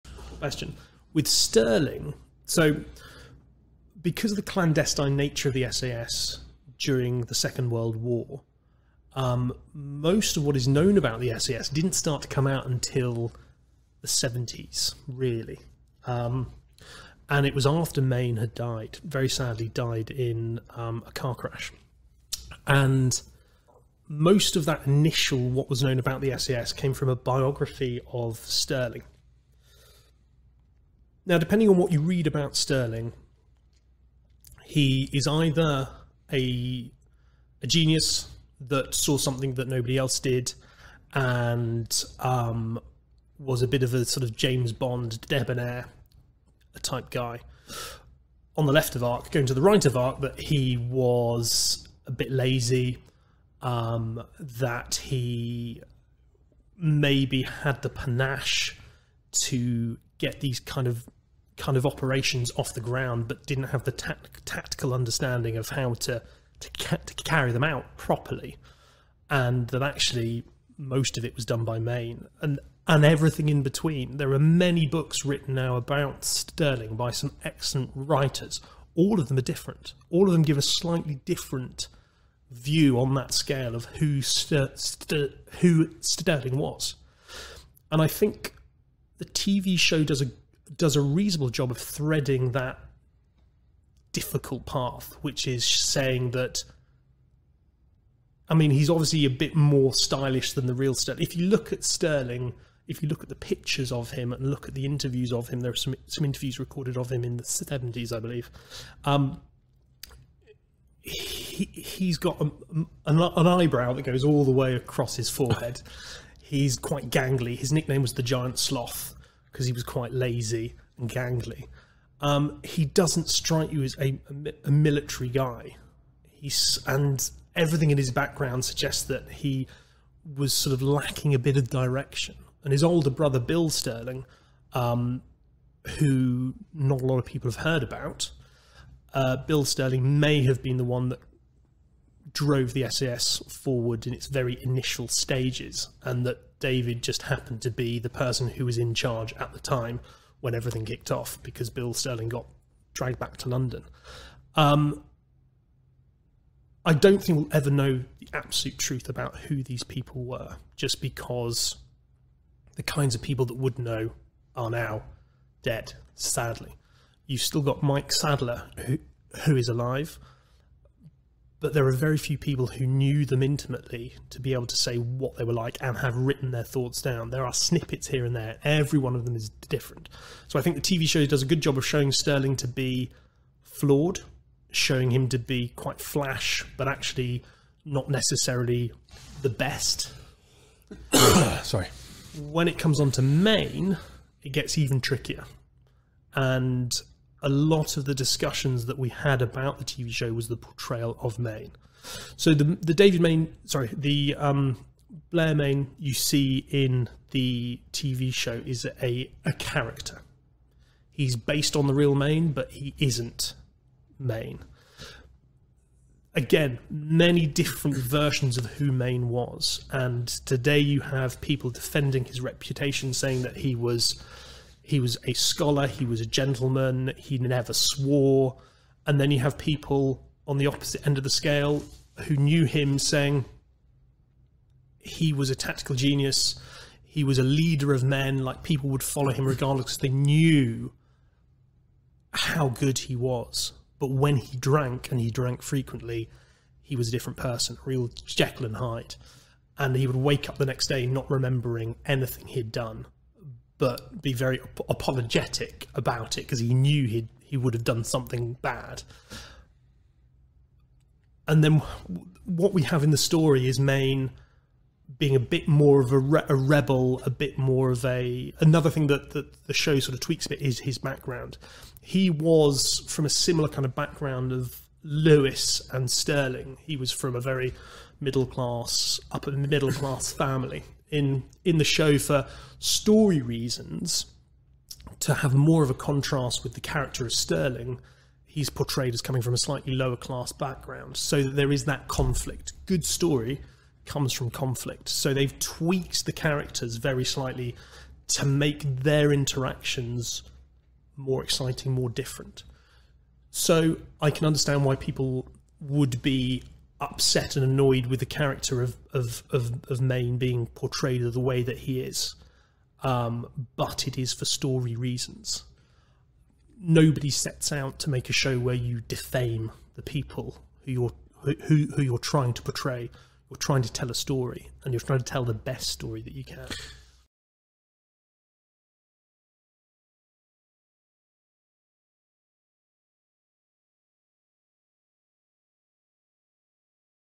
Rogues Heroes portrayal of David Stirling and Paddy Mayne
Rogue Heroes military adviser discuss the on screen portrayal of Stirling and Mayne, compared to historical evidence of their real life personalities.